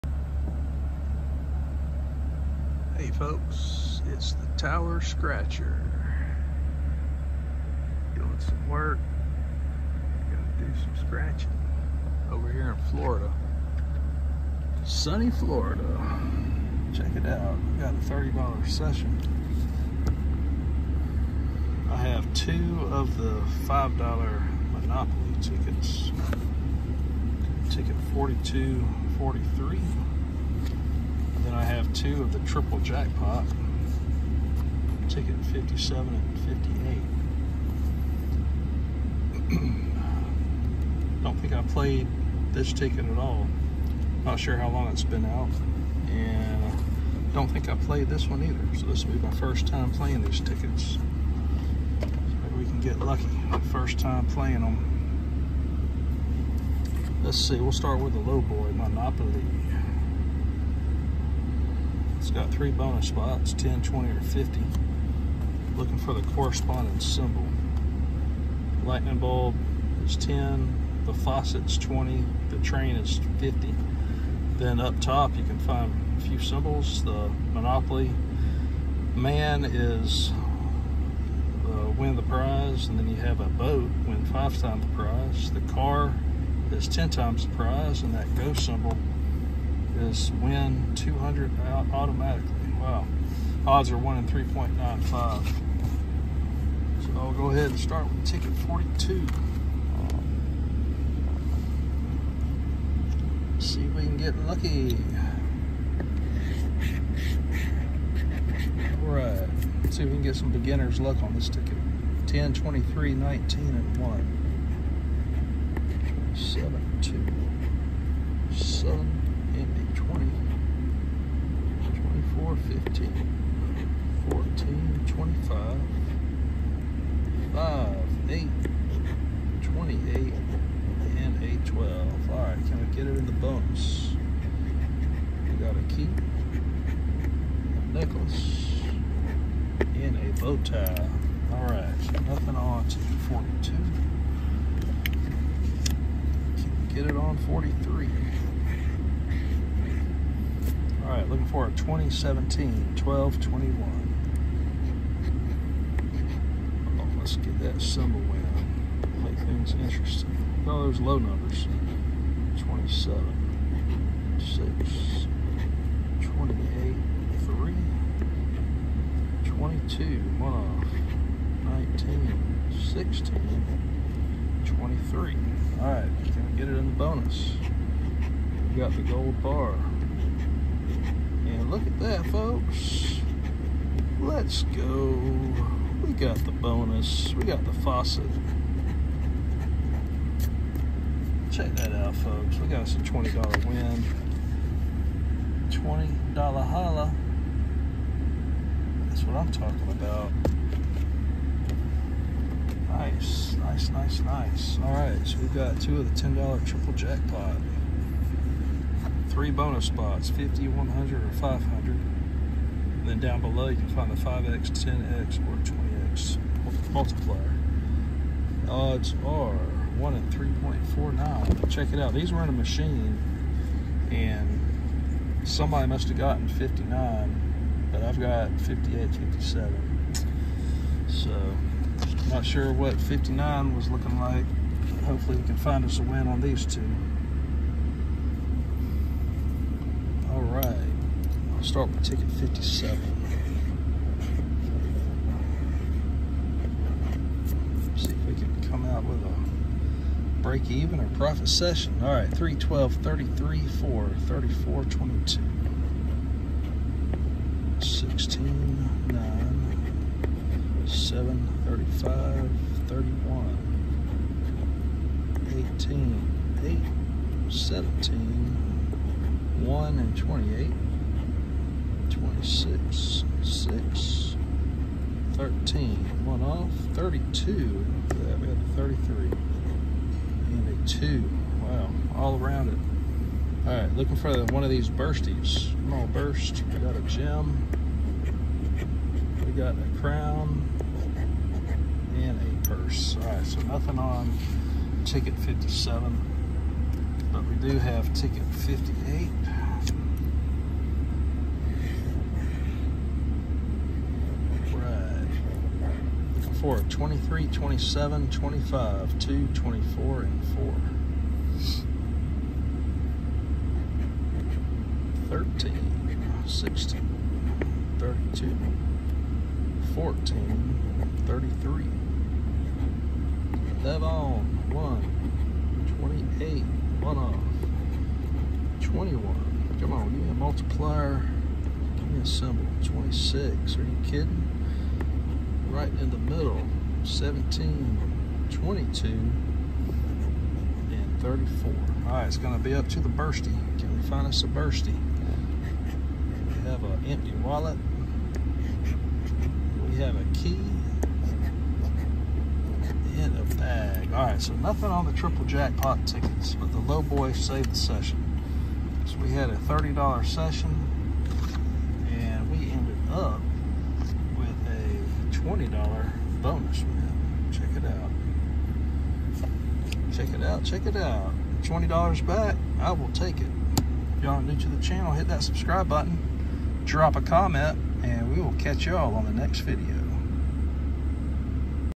New Release! Florida lottery tickets 🦩 Can I find a winner? 🤑 Let’s get it! 🗼
Triple Jackpot
Monopoly Secret Vault
Florida lottery
Gambler nation
Tower scratcher